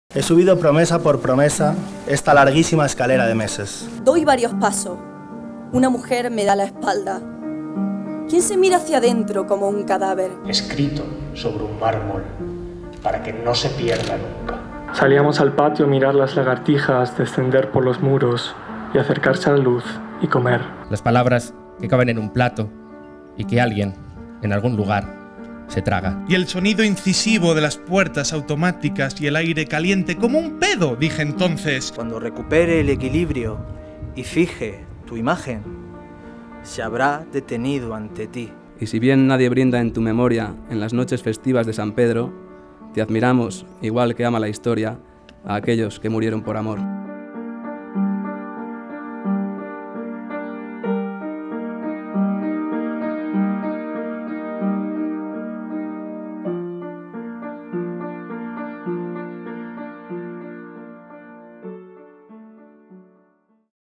Participa en el XIV Premio de Poesía Joven RNE - Fundación Montemadrid
Llega una nueva edición del Premio de Poesía Joven RNE - Fundación Montemadrid, uno de los certámenes poéticos más prestigiosos de nuestro país. y